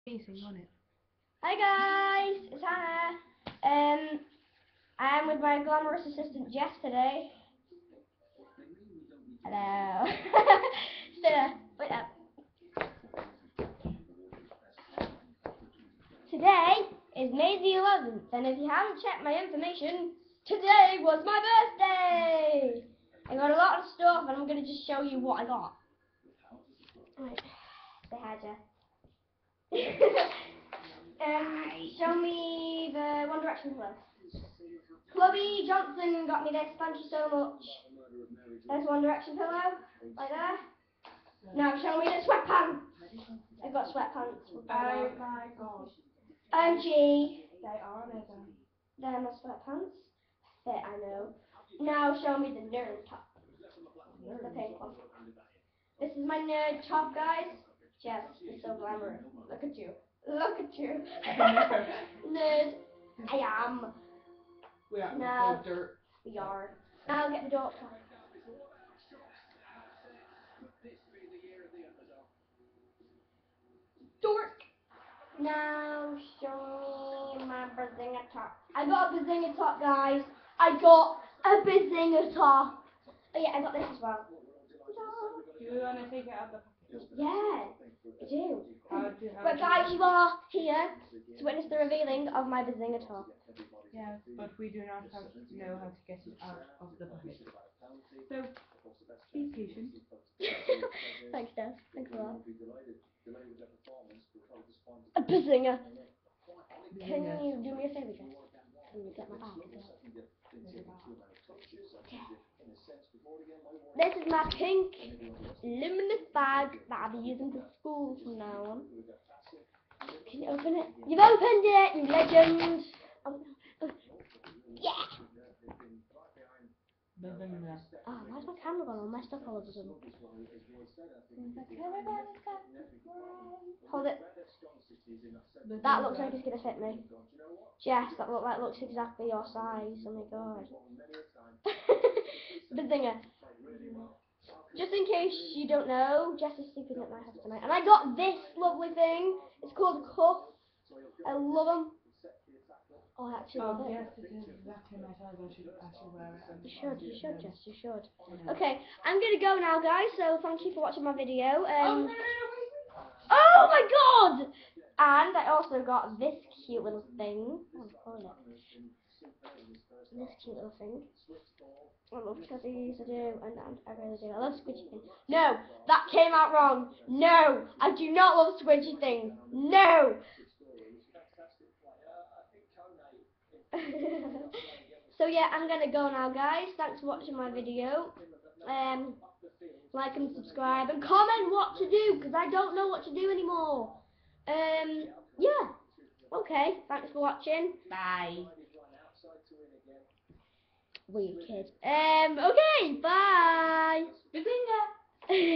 11 May my birthday :D xxxxx
just gonna show you what I got :P with jess brov ;P